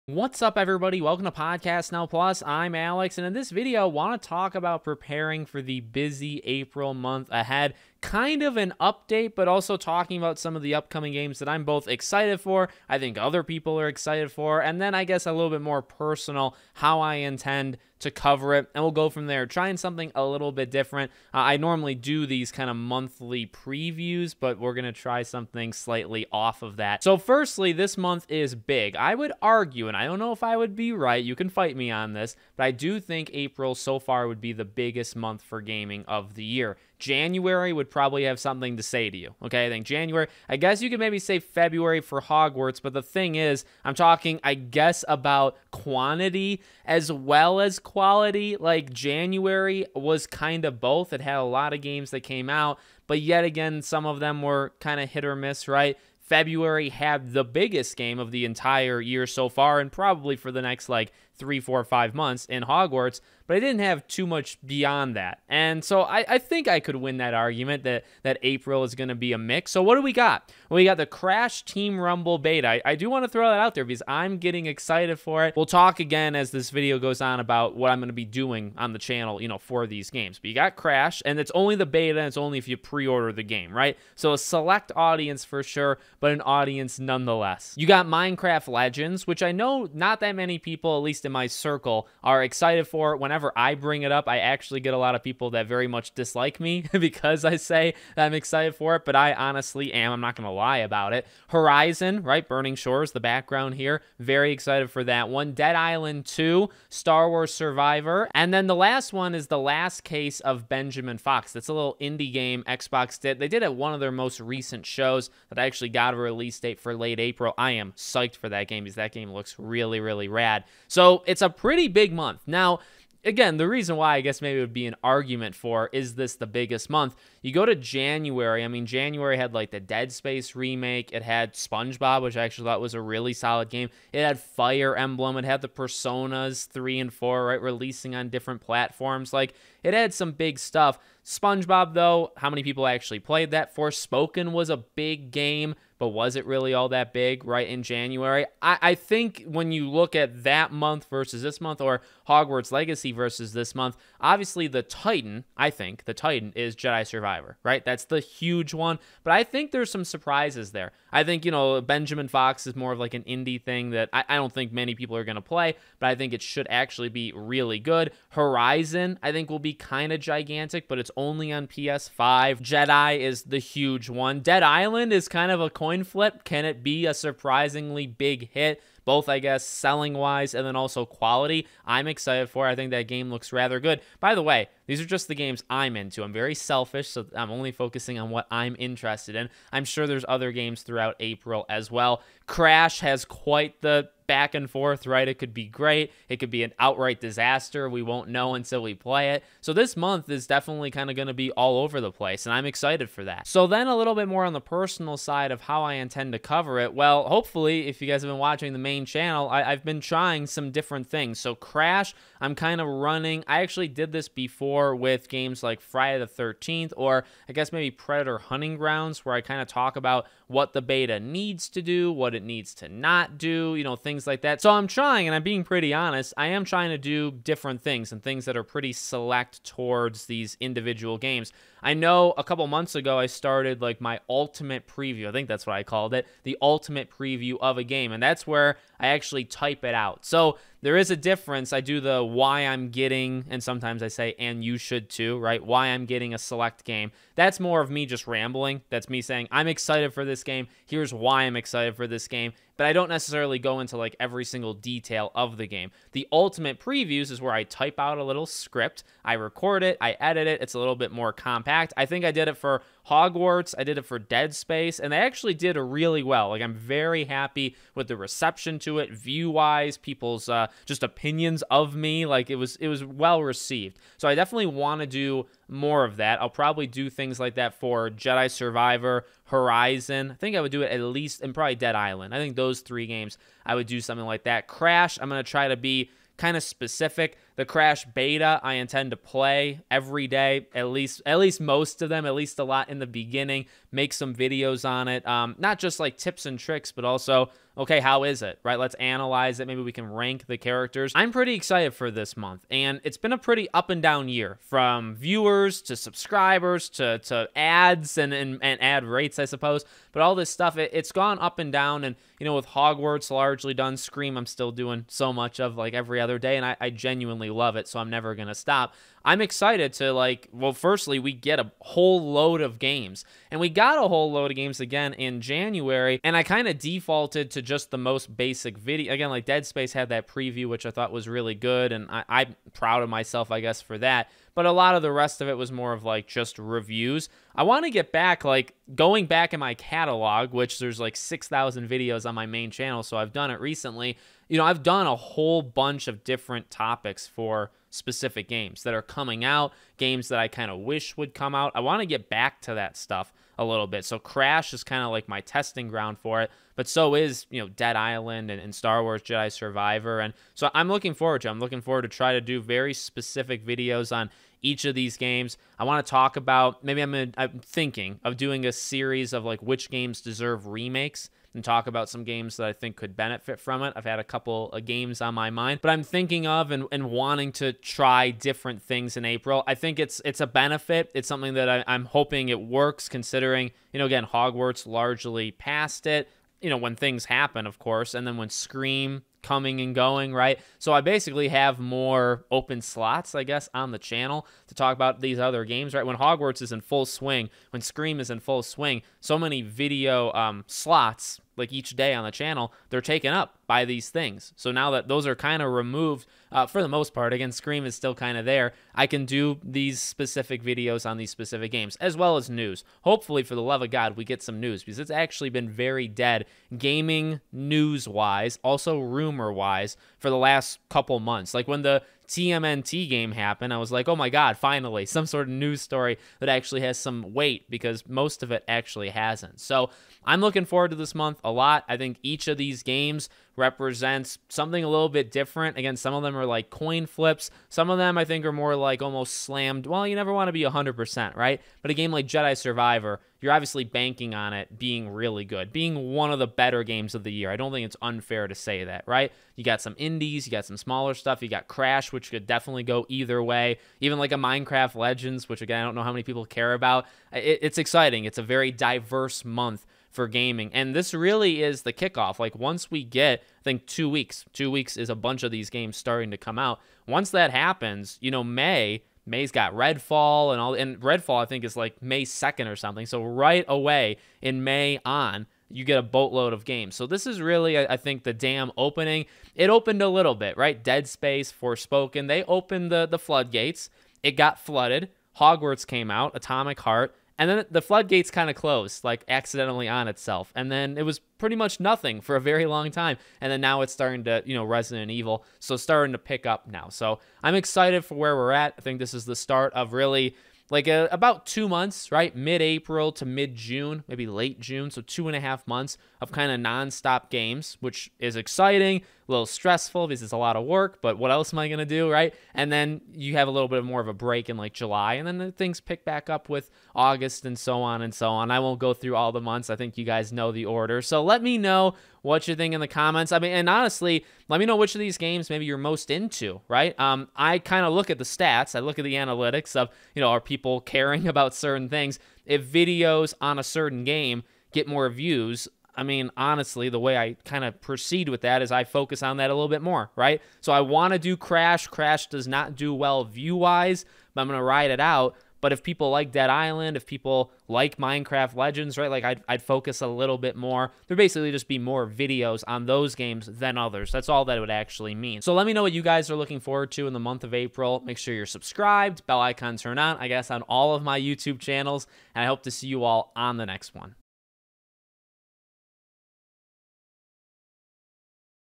Preparing For A VERY Busy April In Gaming
Let's talk about my April Plans for the channel as we approach the busiest gaming month of the year.